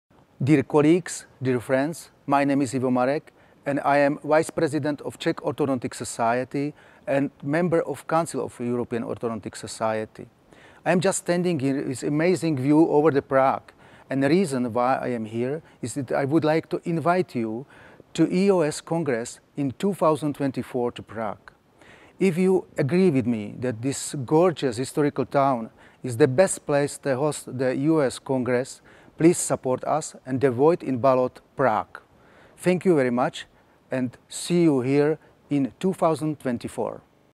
EOS 2024 Prague greetings
Greetings of Dr. Ivo Marek, Ph.D., vice president of the COS.